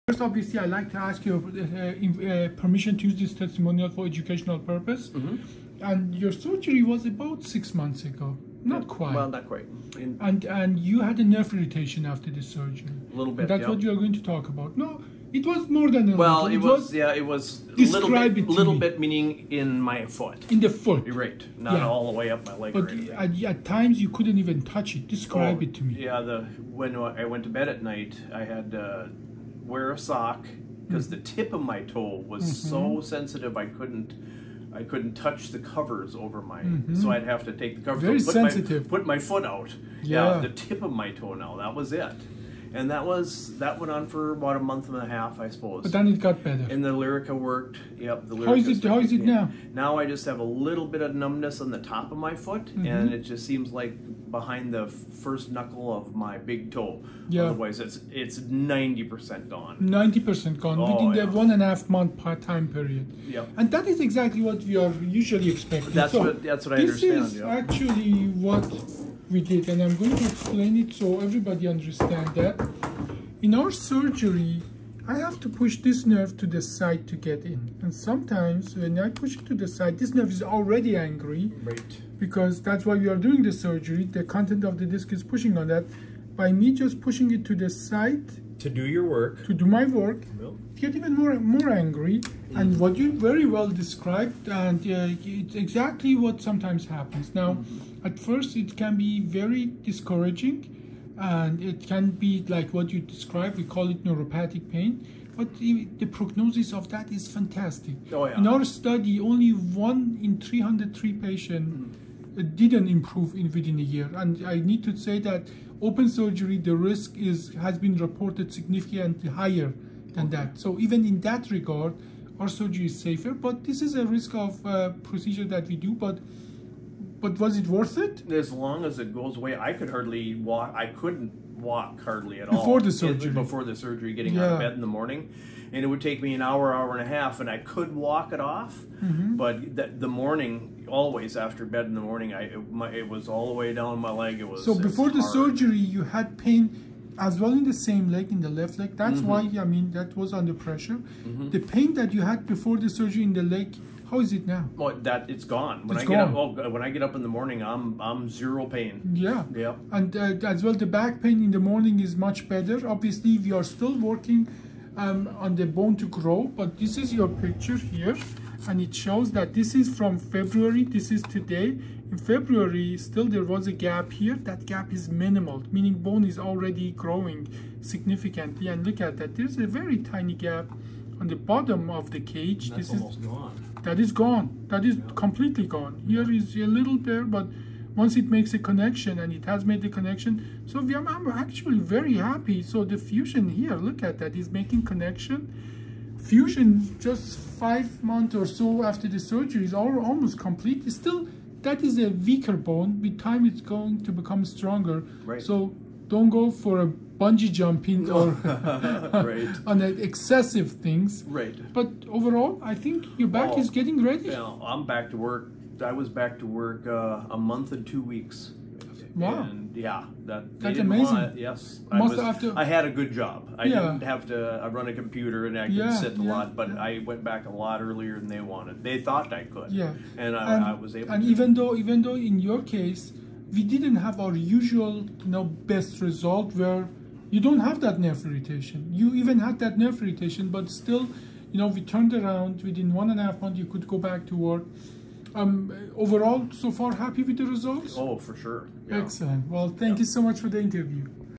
IMG 9110 – Crucial OLLIF nerve irritation explained 90% improved within 6 weeks
6 month post-surgery Let L5 nerve irritation, neuropathic pain – very good prognosis 90% within 6 weeks gone- lecture on anatomy with the nerve irritation is about- previous pain is completely gone – as well how bone grows in CT visualized